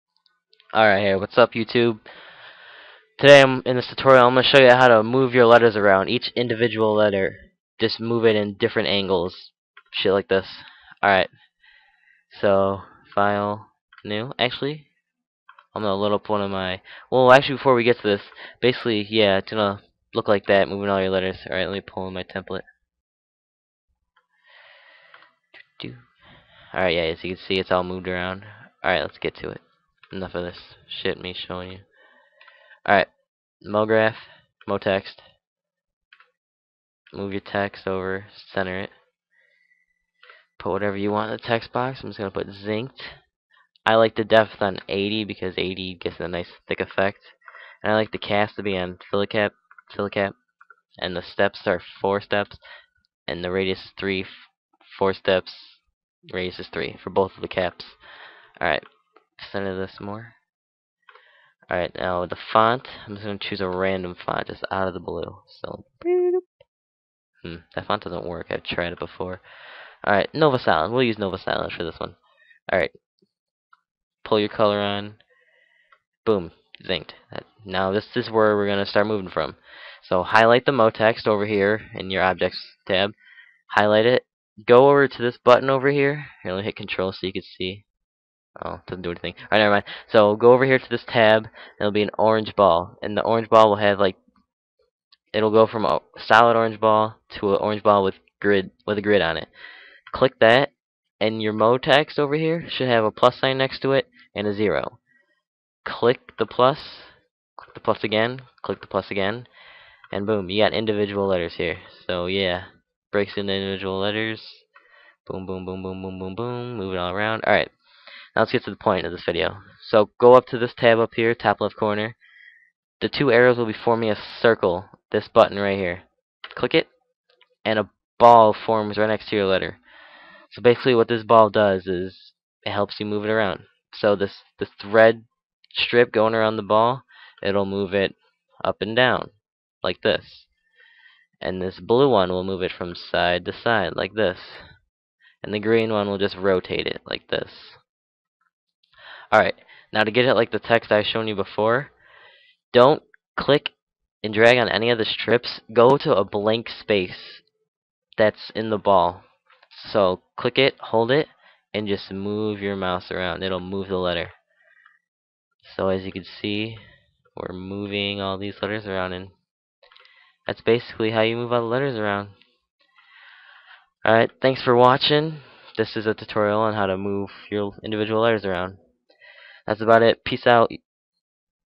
How To Move Individual Letters Around In Cinema4D
In this tutorial i show you how to move individual letters around.